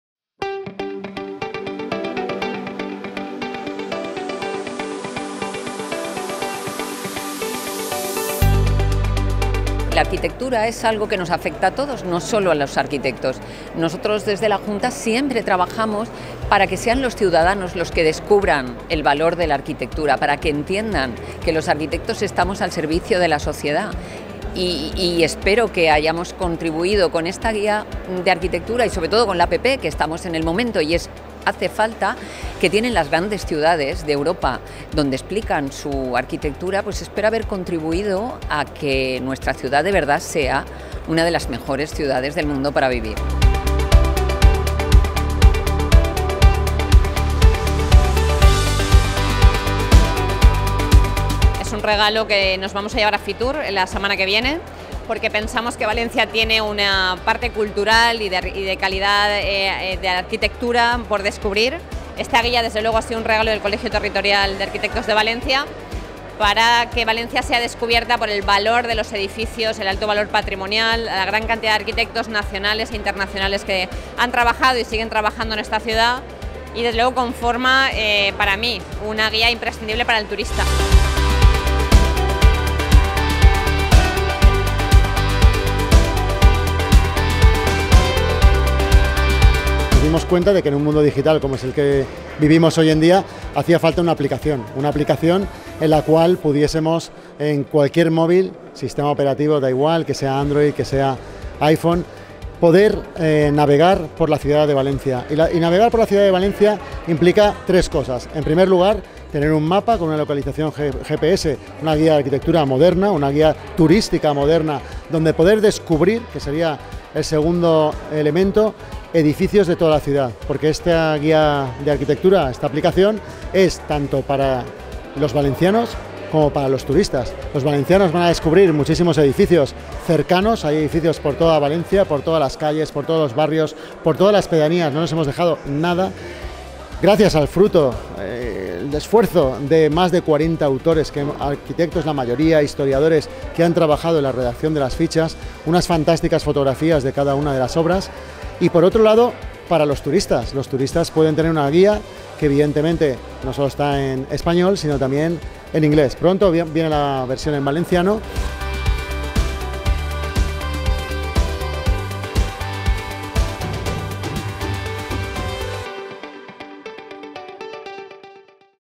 Presentación de la nueva Guía de Arquitectura en el Museo de Bellas Artes de València.
La nueva Guía de Arquitectura cuenta, además de con la publicación en formato libro en el que se plasma la riqueza arquitectónica de la ciudad de València, con una página web y una app